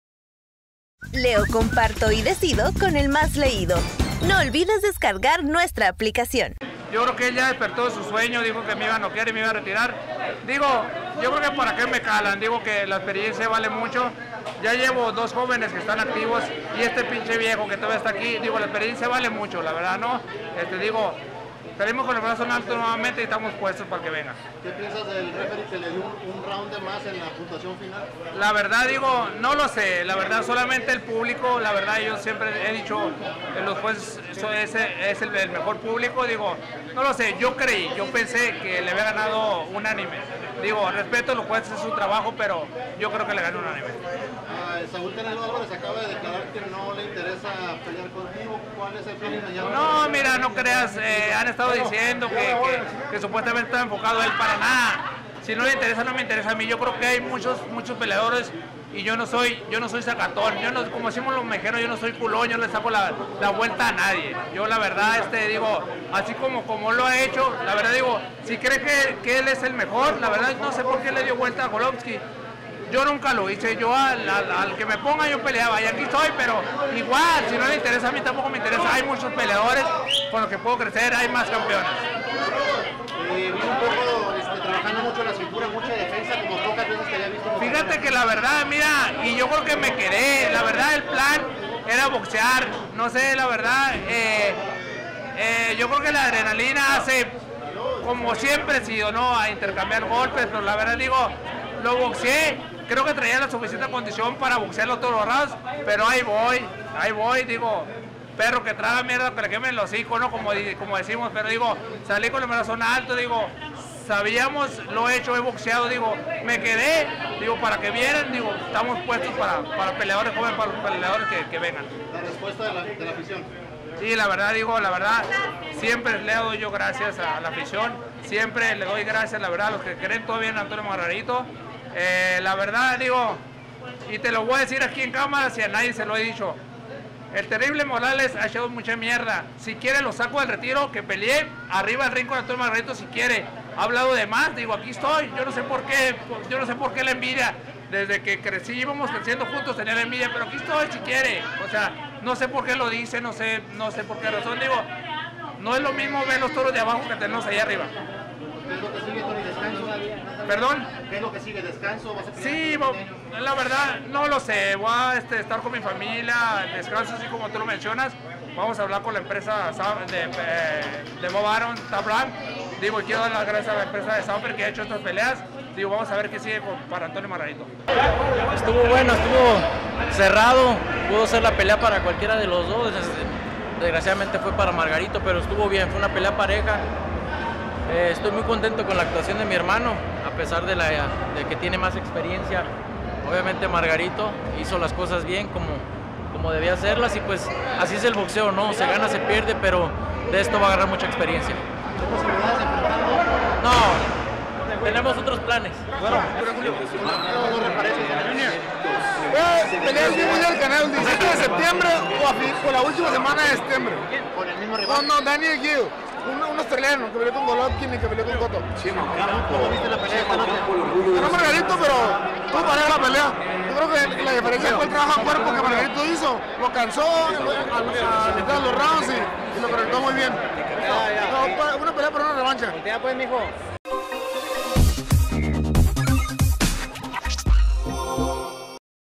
Margarito derribó a Álvarez
"Para qué me calan, la experiencia vale mucho" declaró el ex campeón mundial tijuanense Antonio Margarito al derribar a Ramón “Inocente” Álvarez en el octavo y se apuntó una decisión
dividida, bajo la mirada de Saúl “Canelo” Álvarez, quien estuvo en ringside.